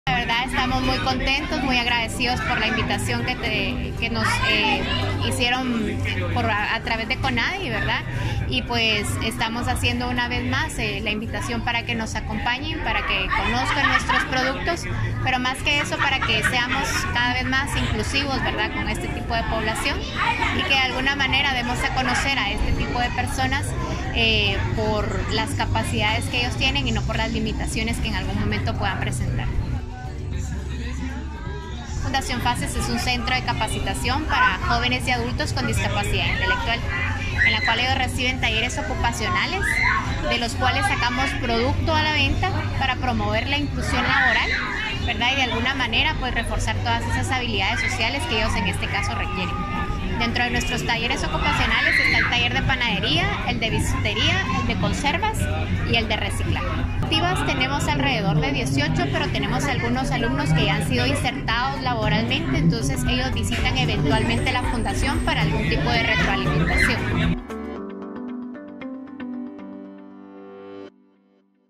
Promueven derechos laborales de las personas con discapacidad
Durante la celebración del 36 aniversario del Mercado Central capitalino, gracias a la gestión que realizó el  Consejo Nacional  para la Atención de las Personas con Discapacidad (Conadi), hoy personas con discapacidad tuvieron la oportunidad de ofrecer productos artesanales para promover el derecho laboral de dicho grupo social.